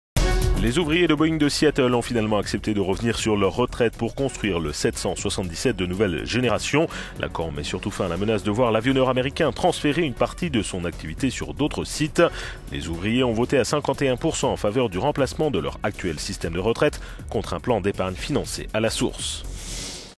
Seattle : les ouvriers de Boeing font des concessions sur leur retraite pour le 777x
Les ouvriers de Boeing de Seattle ont finalement accepté de revenir sur leurs retraites pour...

Les ouvriers de Boeing de Seattle ont finalement accepté de revenir sur leurs retraites pour construire le 777 de nouvelle génération.
L'accord met surtout fin à la menace de voir l'avionneur américain transférer une partie de son activité sur d'autres sites, entraînant la perte de millier d'emploir.